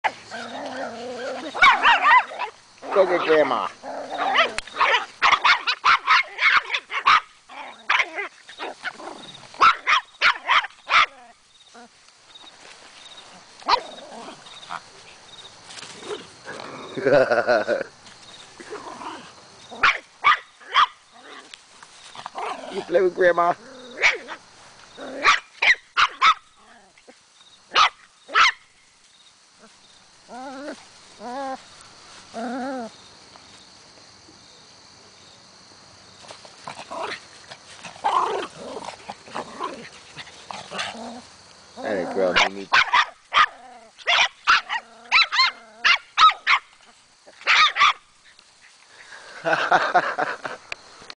Gran'mama Mimi with ZsaZsa's pups
She may look like a pot belly pig but Grandma Mimi can still shuck and jive like Ali.